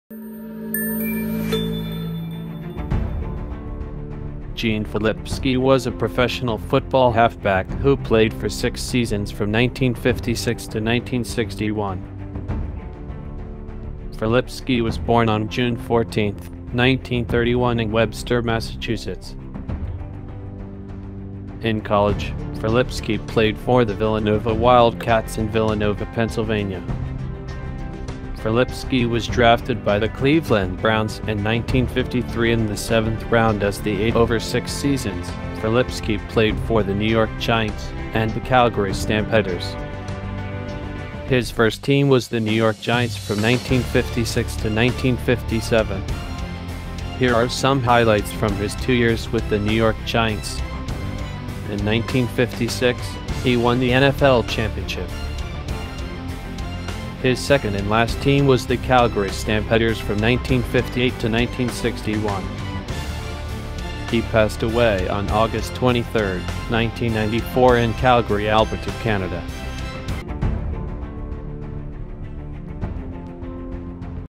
Gene Filipski: Football Halfback
Gene Filipski was a professional football halfback who played for 6 seasons from 1956 to 1961.

In college, Filipski played for the Villanova Wildcats in Villanova Pennsylvania.

Professionally, Filipski played for the New York Giants, and the Calgary Stampeders.

In 1956, he won the NFL championship.